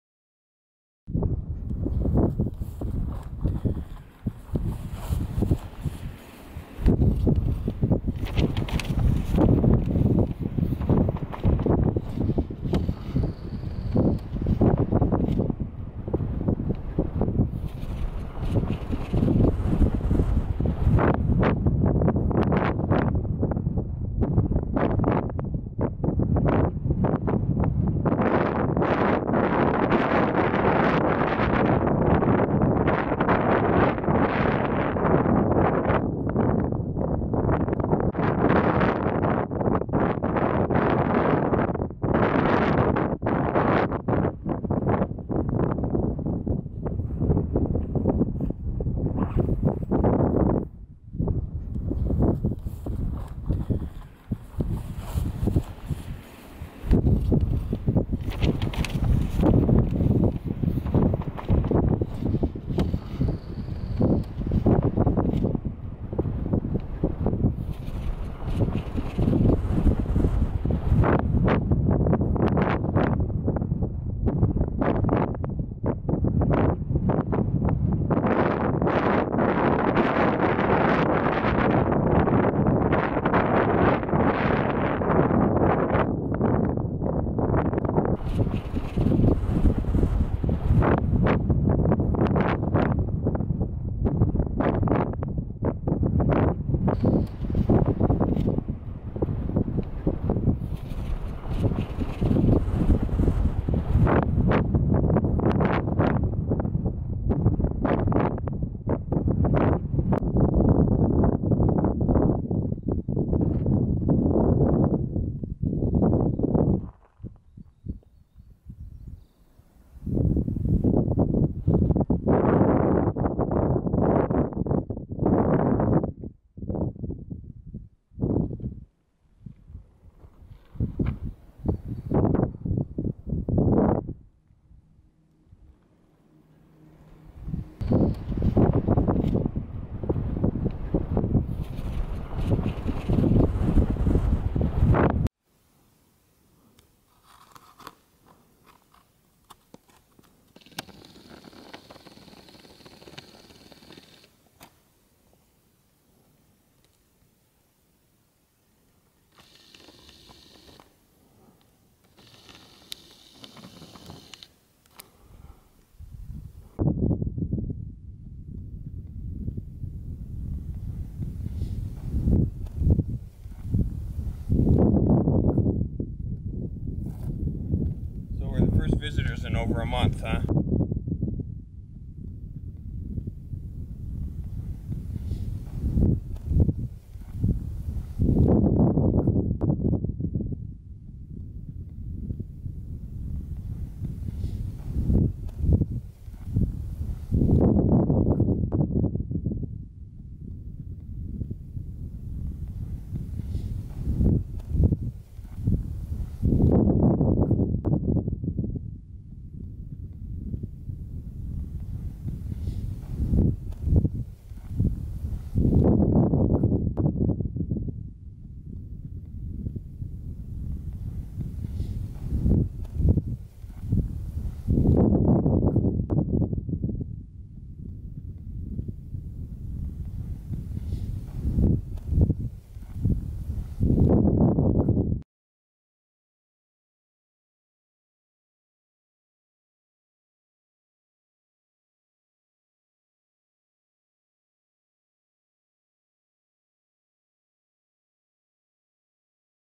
Chaco Culture NHP
A brief look at Chaco and it's history.  Chaco is revered today as a sacred site by many modern-day native people.  Although it is remote, and somewhat difficult to access it provides a unique look at a neglected aspect of North American history and is currently under threat from fracking nearby.